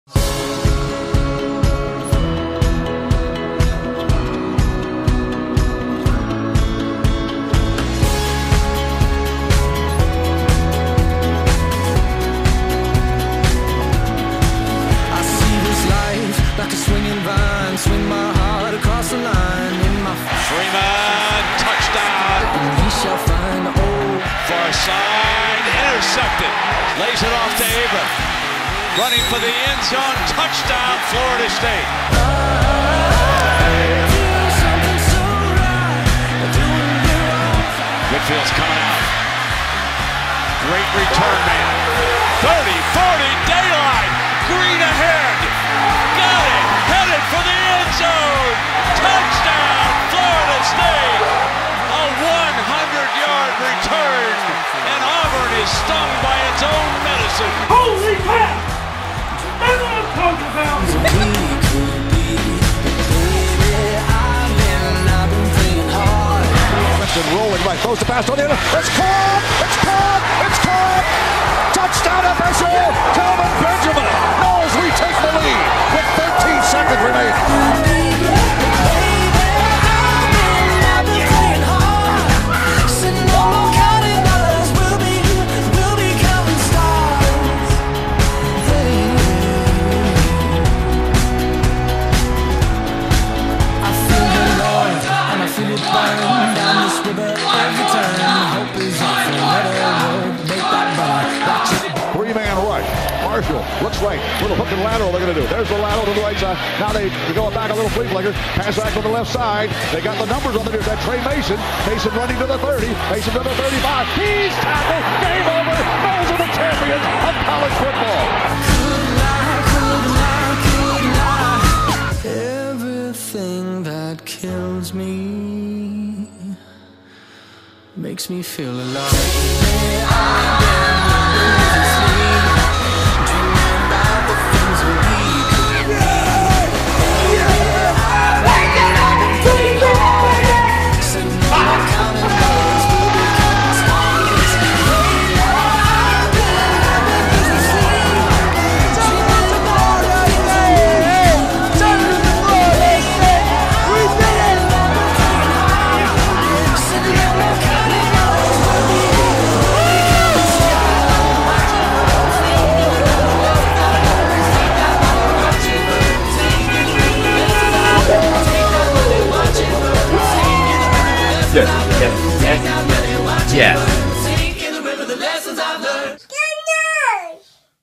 Fans React to Seminoles Winning National Championship
Courtesy Seminole Productions: A look at how fans reacted to the Seminoles winning the BCS National Championship.